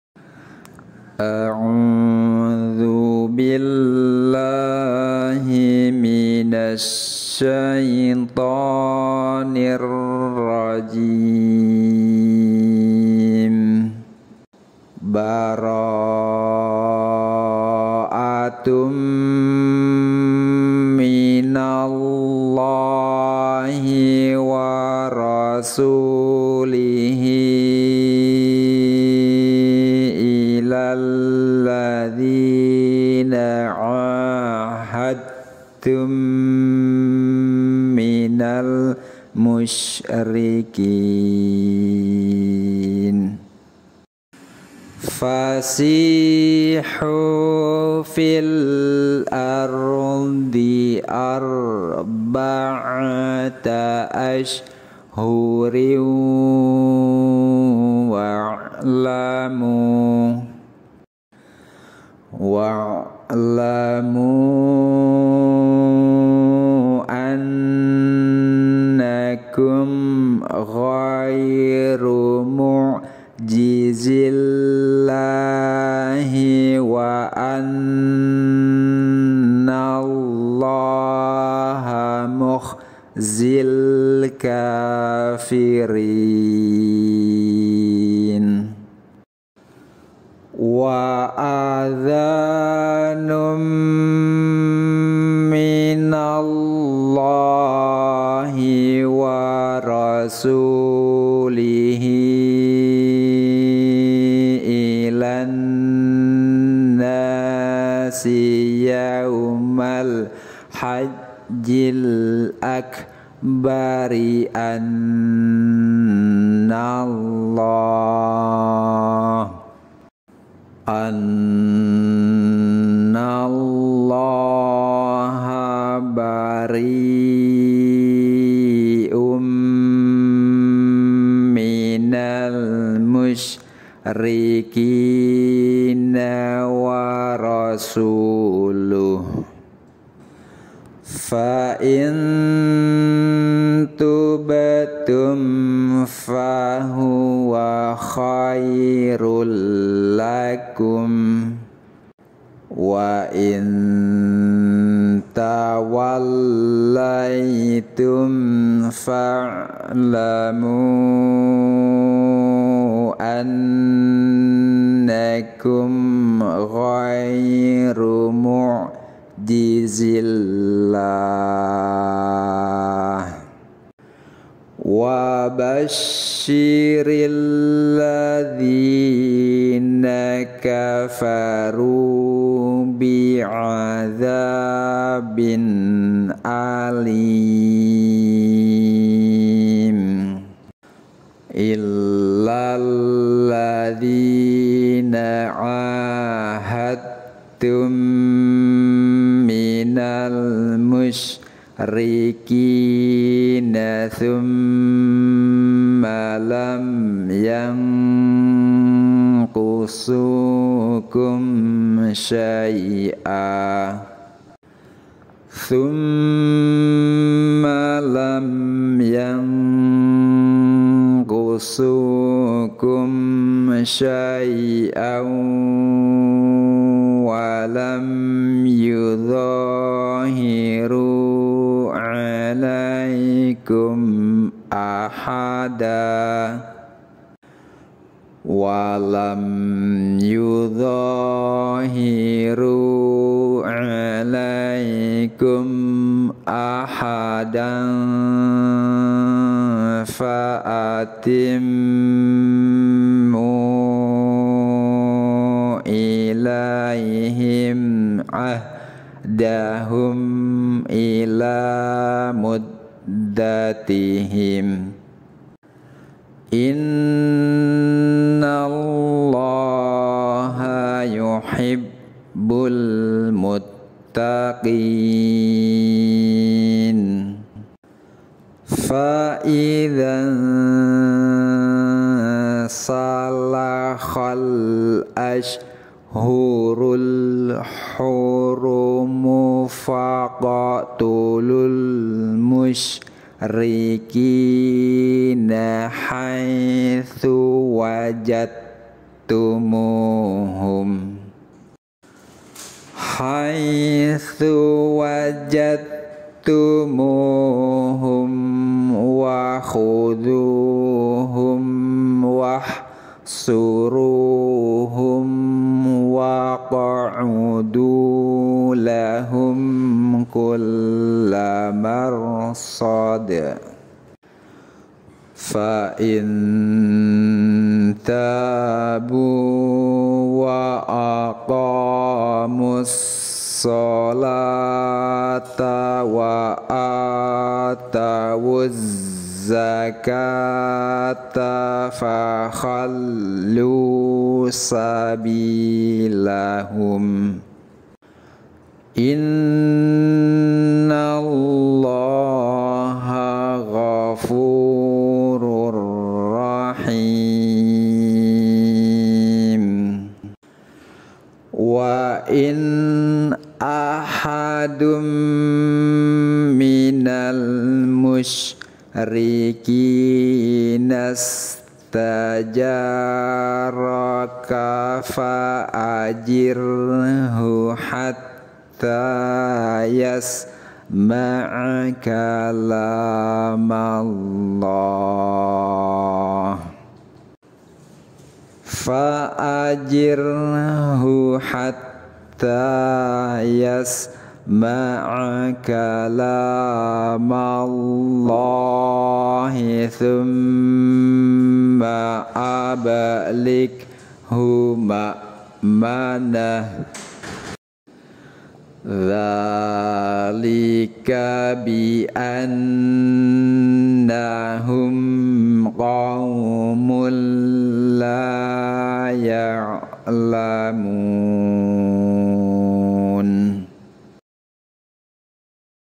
Halaman 187, ngaji Alquran surah Al Baroah / At Taubah ayat 1-6 untuk pemula
Halaman 187, ngaji Alquran surah Al Baroah / At Taubah ayat 1-6 untuk pemula yang baru belajar membaca Alquran sesuai kaidah tajwid. Bacanya pelan-pelan agar mudah disimak dan diikuti.